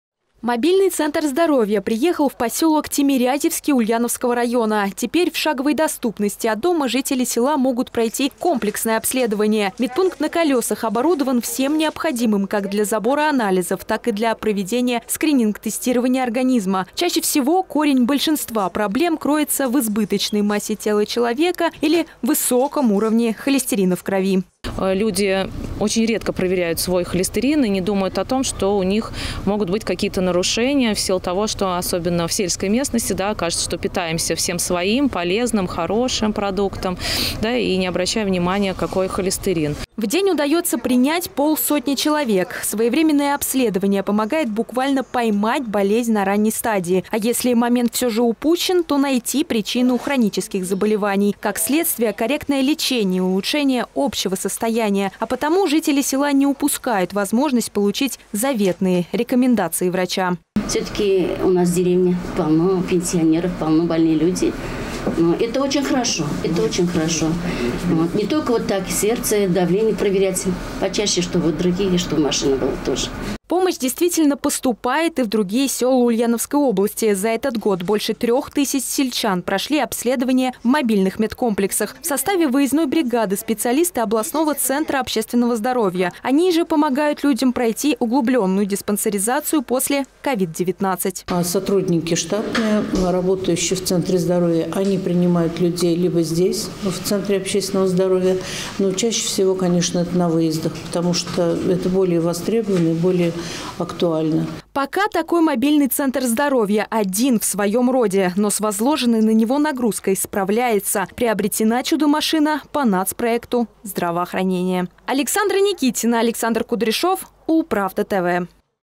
Один на всю область. Как в Ульяновске мобильные центры здоровья работают
Еще один шаг к доступной медицине – мобильные центры здоровья. Только за этот год организовано больше 60 выездов в отдаленные села Ульяновской области. Это позволило обследовать три тысячи жителей. Внутренняя начинка машины не уступает передовым поликлиникам города, что позволяет получать качественную медпомощь во всех уголках региона.